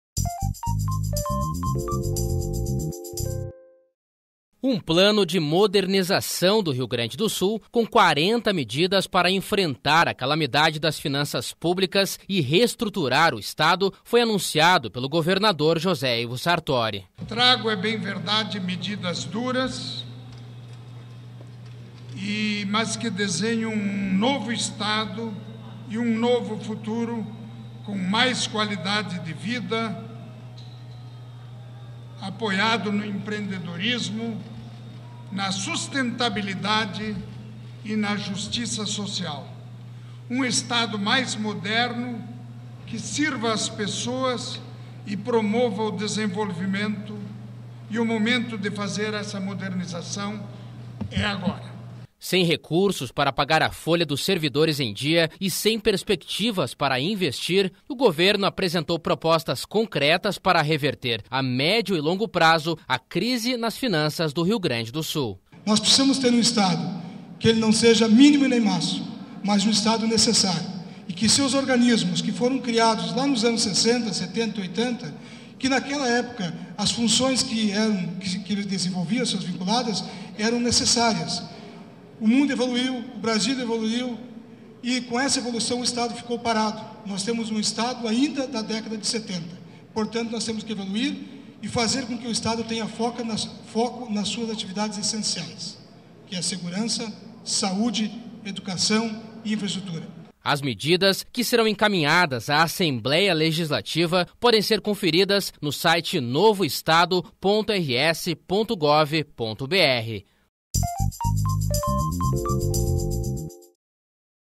Medidas modernizam Estado para enfrentar calamidade das finanças públicas
Um plano de modernização do Rio Grande do Sul, com 40 medidas para enfrentar a calamidade das finanças públicas e restruturar o estado, foi anunciado pelo governador José Ivo Sartori.